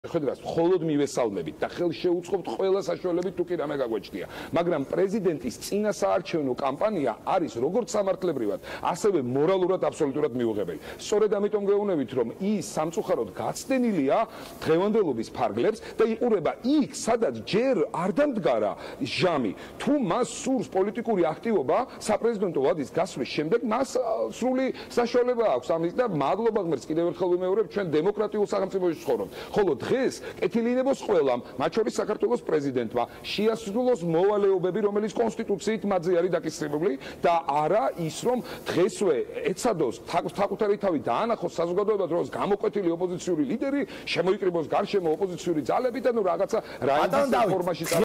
დავით მათიკაშვილი ლევან ჩხაიძის დღის ამბებში
პრეზიდენტის წინასაარჩევნო კამპანია როგორც სამართლებრივად, ისე მორალურად აბსოლუტურად მიუღებელია - დავით მათიკაშვილი